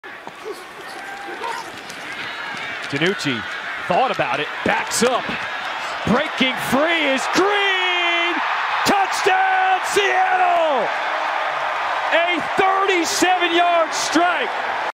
Touchdown pass WHILE RUNNING BACKWARDS!
Seattle Sea Dragons Quarterback Ben DiNucci scrambles out of the pocket for this touchdown pass to Juwan Green